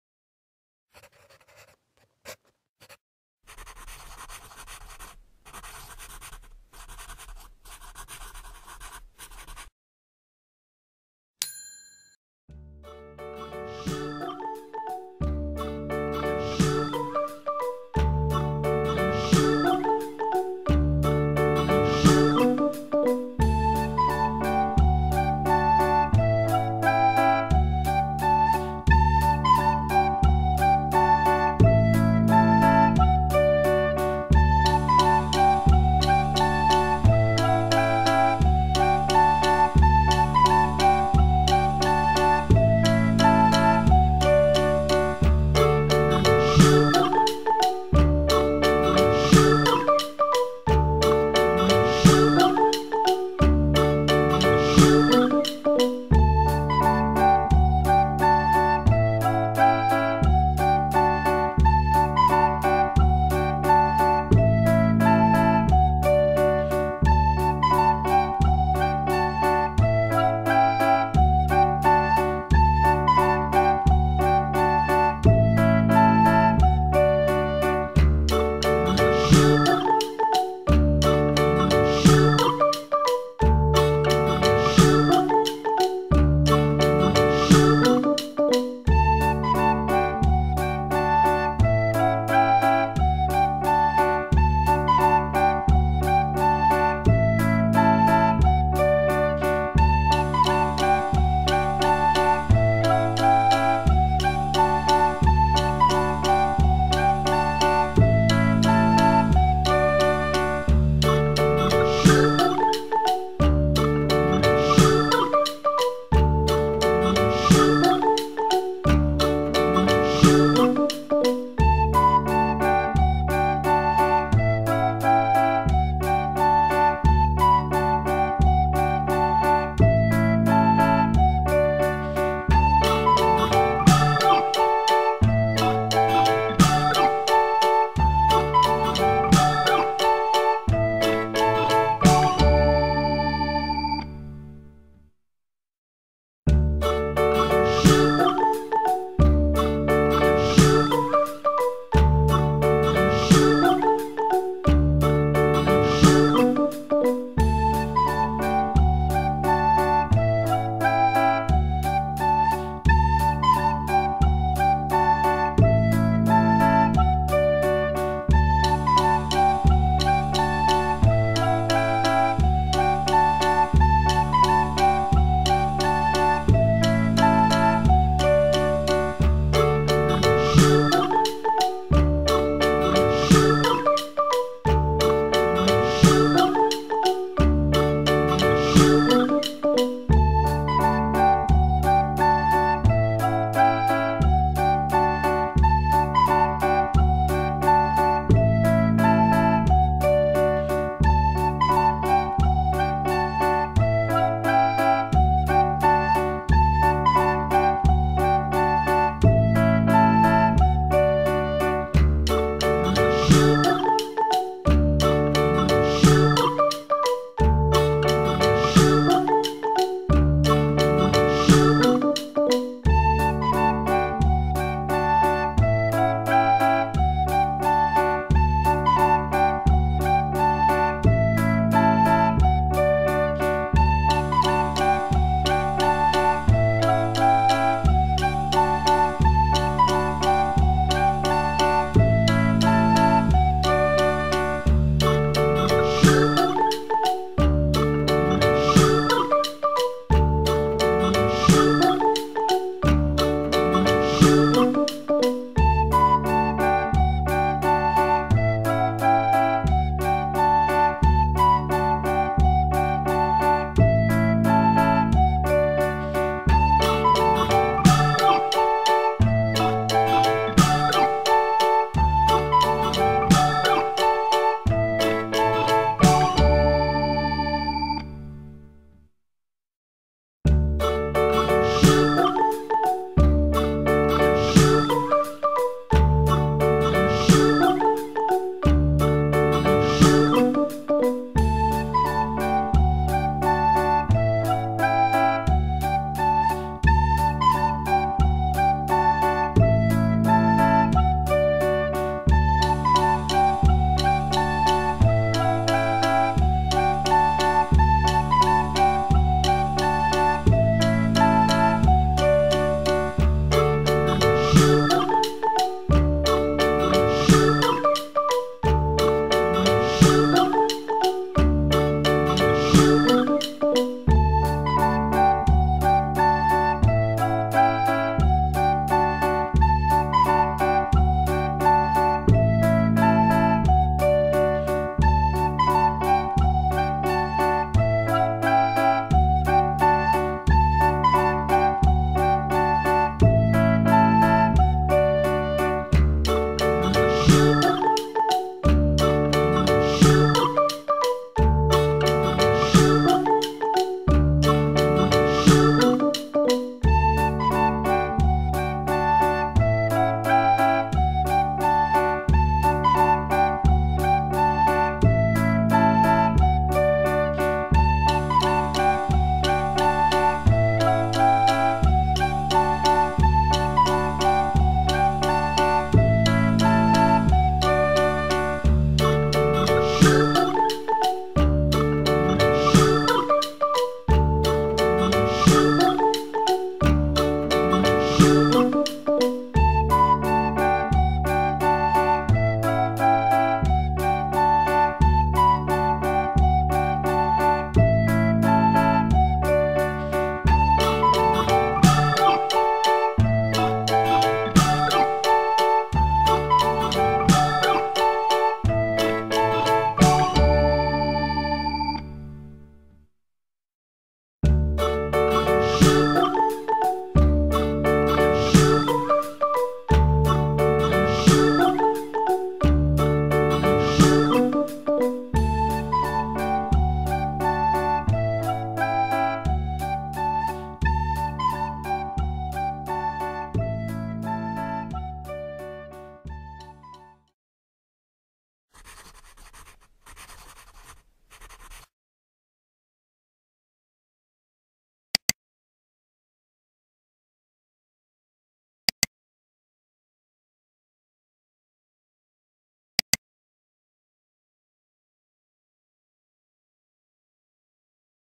How to Draw Donald Duck Easy Step by Step
Easy step by step tutorial on how to draw a motorcycle, pause the video at every step to follow the steps carefully. Enjoy ;)